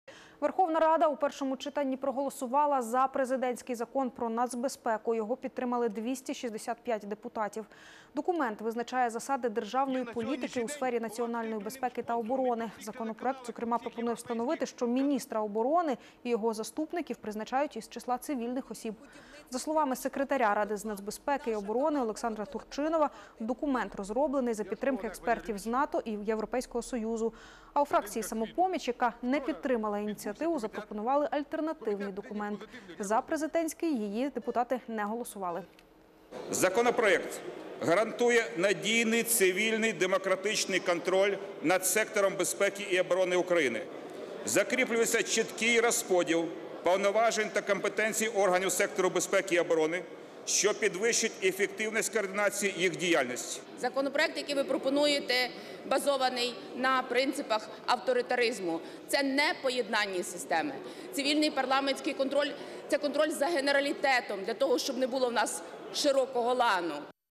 Верховна Рада у першому читанні проголосувала за президентський закон про нацбезпеку
Його підтримали 265 депутатів. Документ визначає засади державної політики у сфері національної безпеки та оборони. Законопроект, зокрема, пропонує встановити, що міністр оборони і його заступники призначаються із числа цивільних осіб. За словами секретаря Ради з нацбезпеки і оборони Олександра Турчинова – документ розроблений за підтримки експертів з НАТО і Європейського Союзу. А у фракції "Самопоміч", яка не підримала ініціативу, запропонували альтернативний документ. За Президентський не голосували.